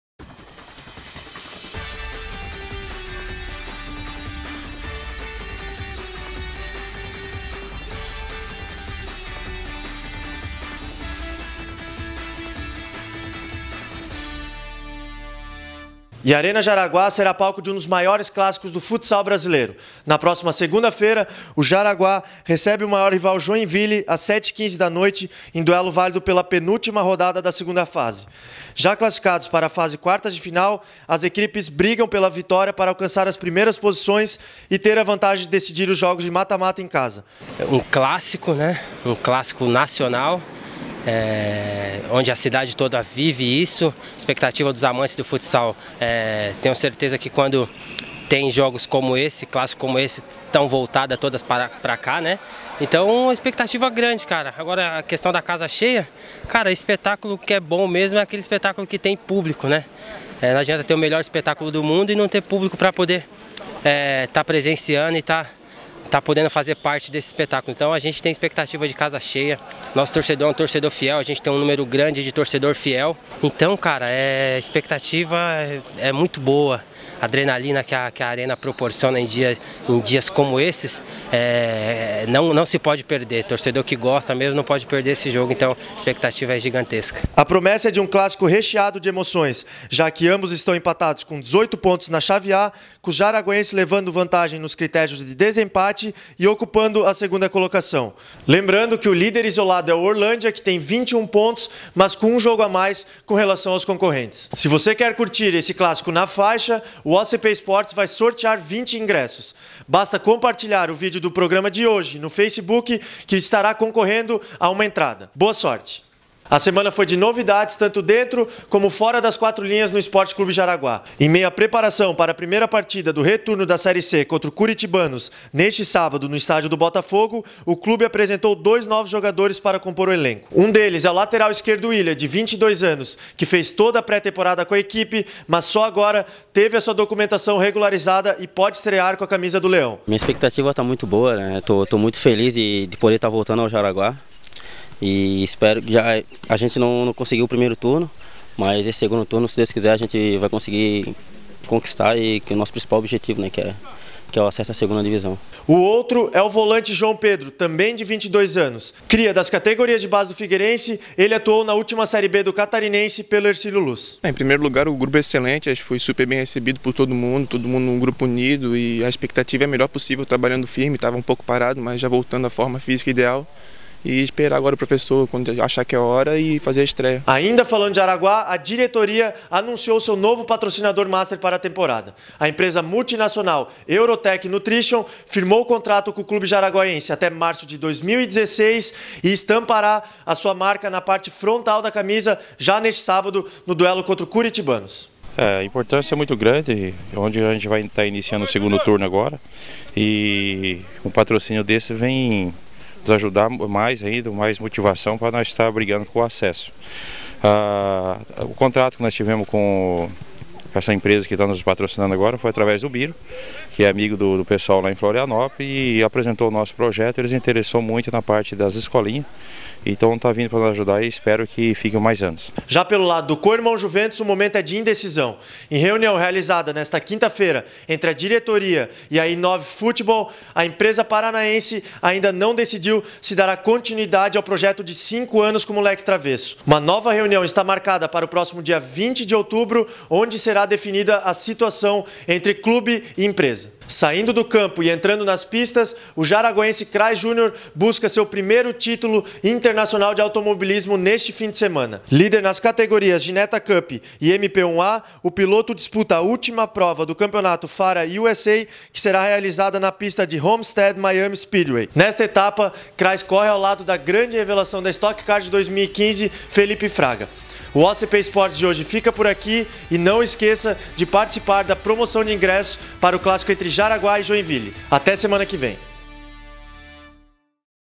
OCP Esportes - Maior clássico do futsal promete lotar a Arena Jaraguá (sorteio de ingressos)
Além do clássico, destacamos o SC Jaraguá que traz novos reforços para o time principal e fecha patrocínio master para a temporada 15/16.
O piloto jaraguaense Kreis Jr, disputa o título inédito no campeonato FARA USA.
O futuro indefinido da parceria I9 e Juventus.